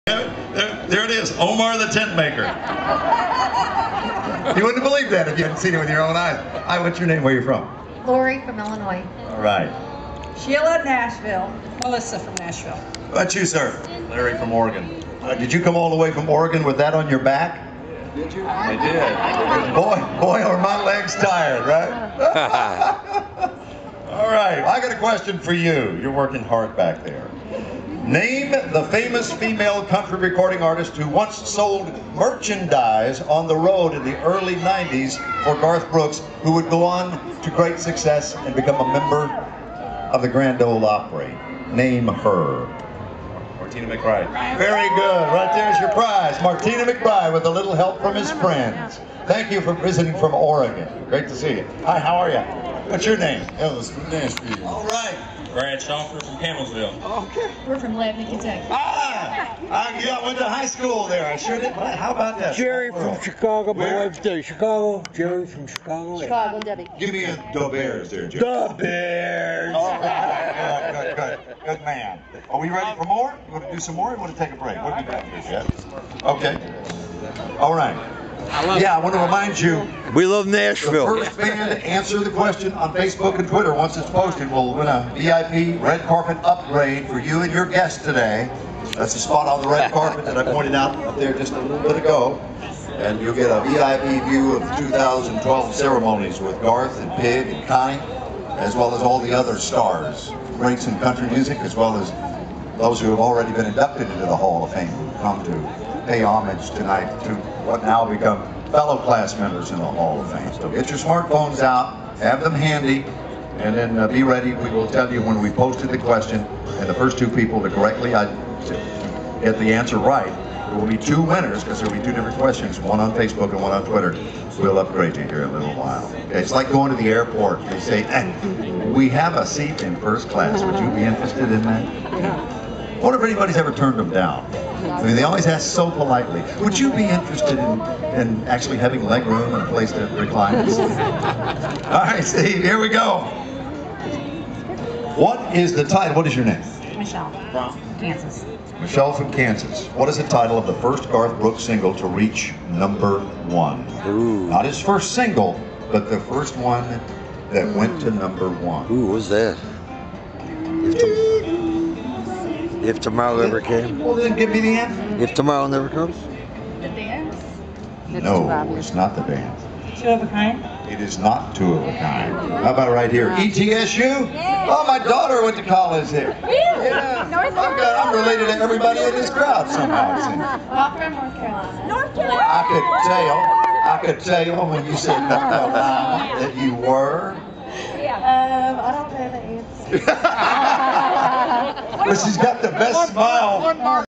Garth Brooks pre-induction into the Country Music Hall Of Fame (pt 1)
Garth Brooks per-induction into the Country Music Hall of Fame (Oct 21, 2012 Nashville, Tenn.